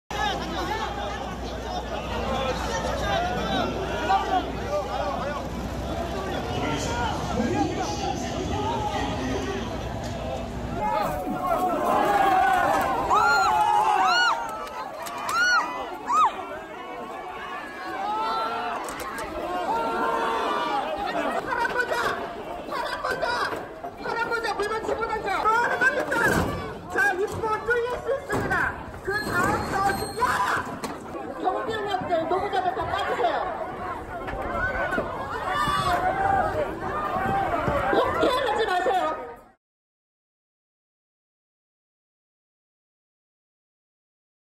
'밀치고 넘어지고'… 옛 노량진 수산시장 강제집행 과정서 '격한 충돌'
서울중앙지법과 수협중앙앙회가 25일 오전 10시10분부터 서울 동작구 옛 노량진 시장의 전체 판매자리와 부대·편의시설 119개소에 대한 명도 강제집행에 나서며 시장 상인들과 충돌하고 있다.

영상 촬영 : 최동수 기자, 김지성 인턴기자
영상 편집 : 김소영 기자

취재현장의 따끈한 영상 놓치지 마세요